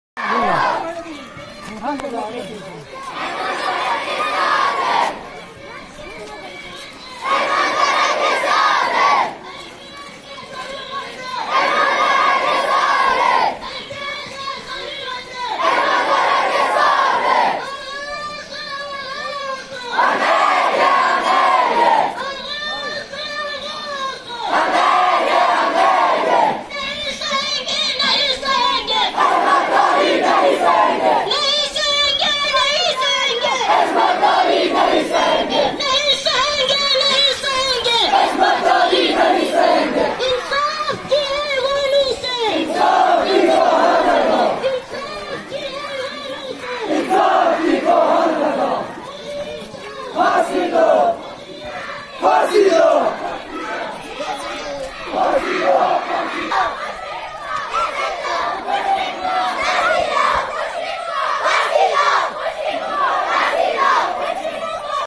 Sumbal | Rape | Case |14-05-2019
MPHS SALISKOTE marks protest against the culprit and shows sympathy with the victi and demands stern action against the culprit.